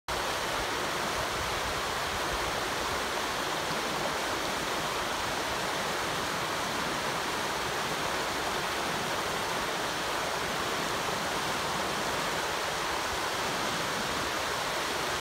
Oak Creek/West Fork Oak Creek flow event March 4, 2019
Oak Creek/West Fork Oak Creek confluence during spring flow event.  Oak Creek is in background and West Fork is in foreground. Adjacent to Yavapai County ALERT Gauge 155 (West Fork Oak Creek).